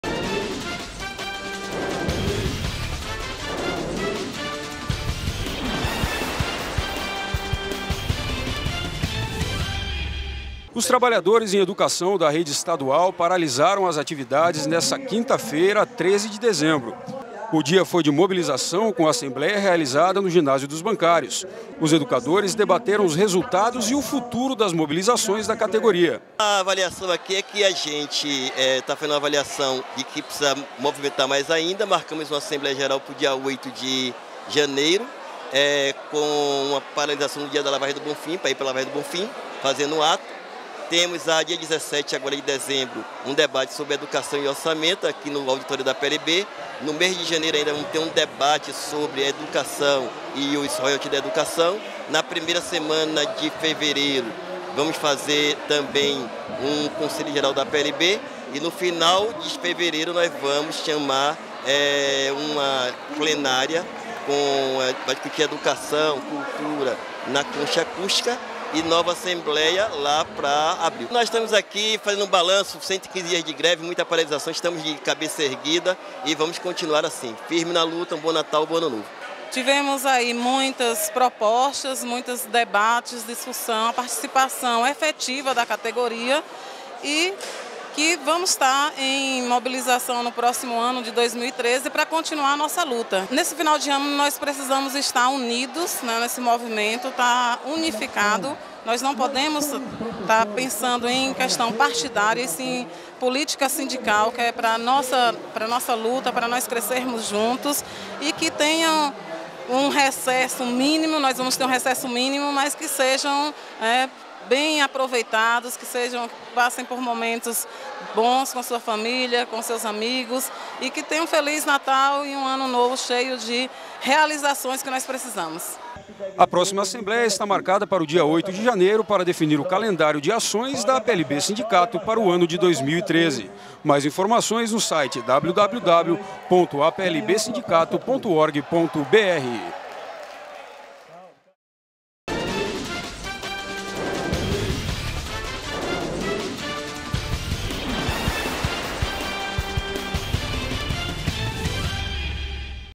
TV APLB - APLB-Sindicato realiza sua última assembleia do ano
Trabalhadores em Educação realizaram assembleia nesta quinta, dia 13 de dezembro, no ginásio dos bancários.